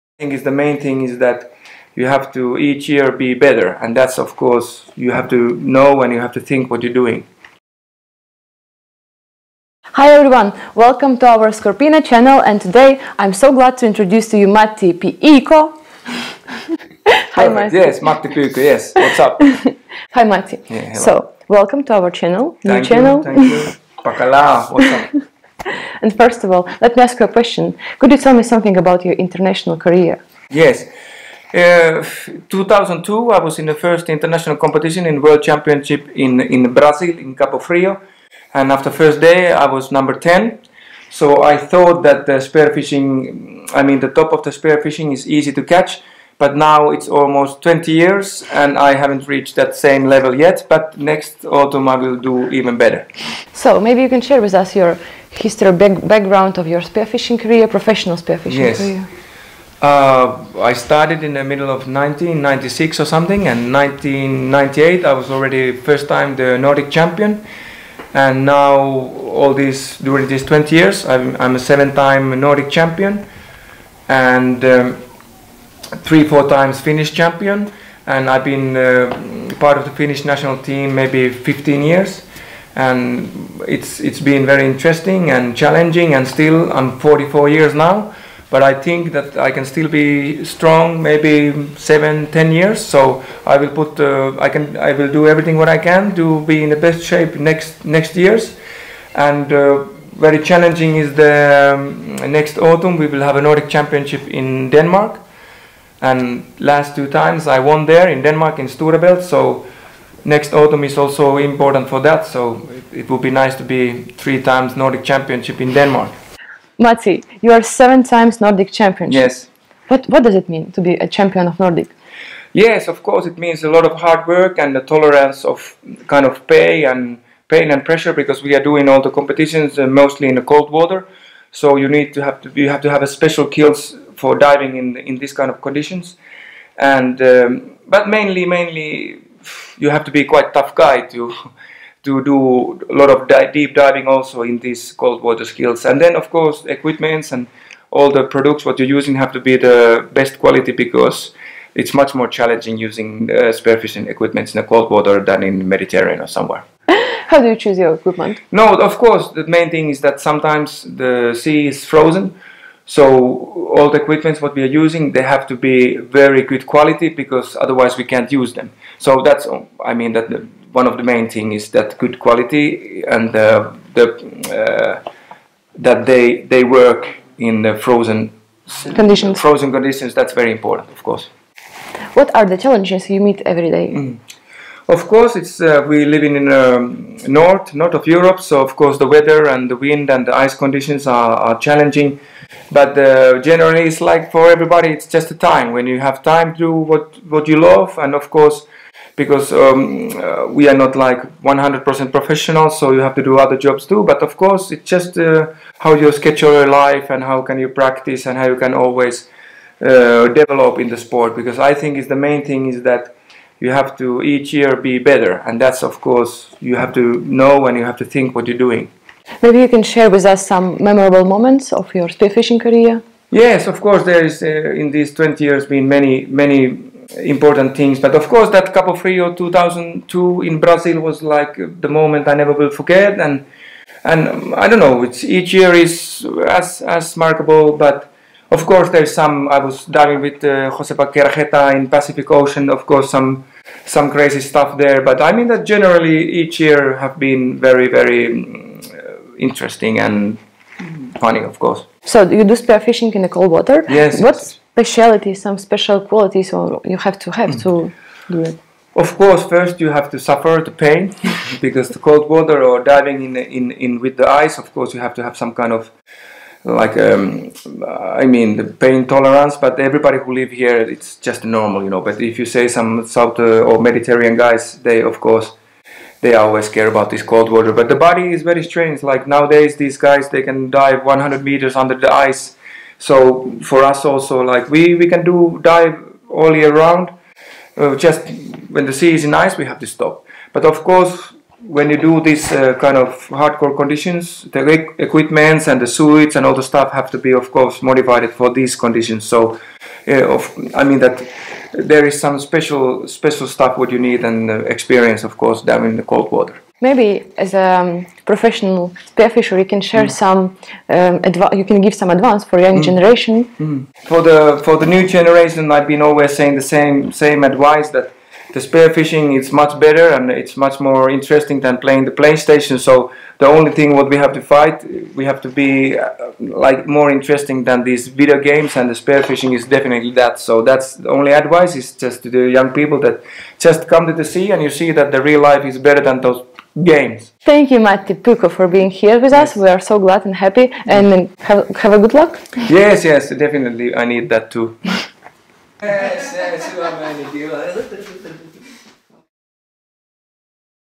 Mattyy Pyykko about his International Career
Matty Pyykko - Leading Ambassador of SCORPENA. For twenty years of spearfishing career, Matti seven times became the Nordic Champion, won the @AtlanticChampionship in 2015, regularly won @ Kristiansundcup competitions in Denmark and many more...